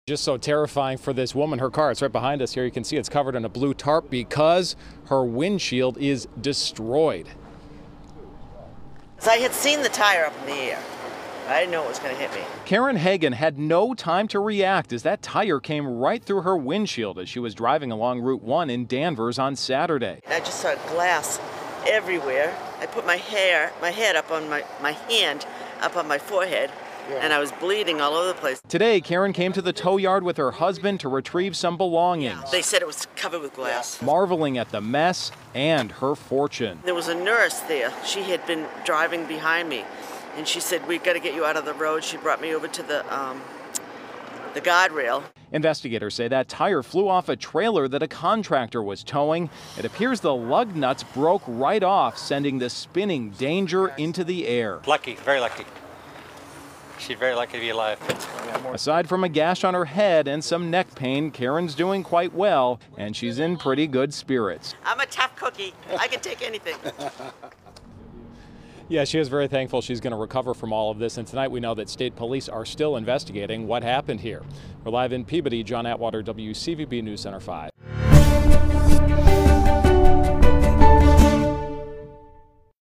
Tire smashes through driver’s windshield on Route 1 in Danvers
The woman behind the wheel was hurt, but she thankfully suffered non-life-threatening injuries.